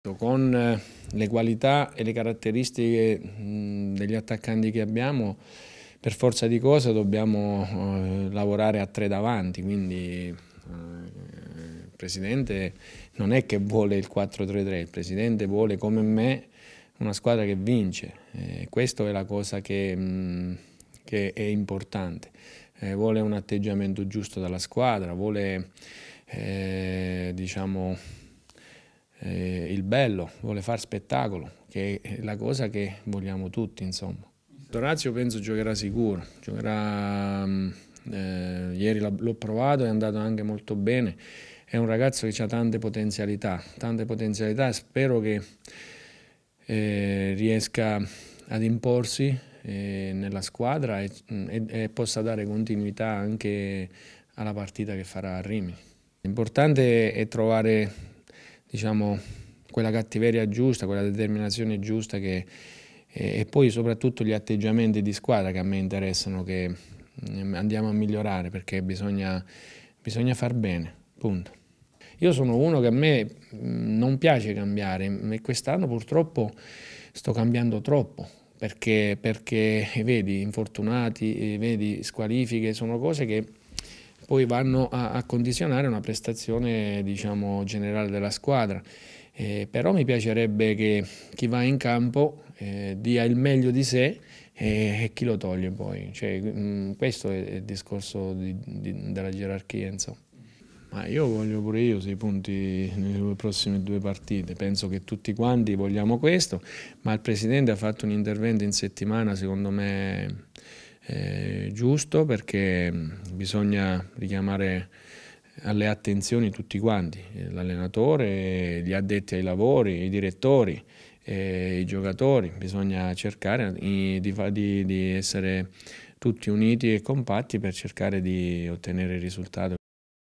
Vivarini: "Anch'io voglio tre punti col Rimini"
Il tecnico del Teramo Vivarini riprende così le parole pronunciate a inizio settimana dal presidente Campitelli. Intanto il difensore Brugaletta recupera dal problema ad una caviglia ed è convocato per domani. Assenti Perrotta, Altobello e Speranza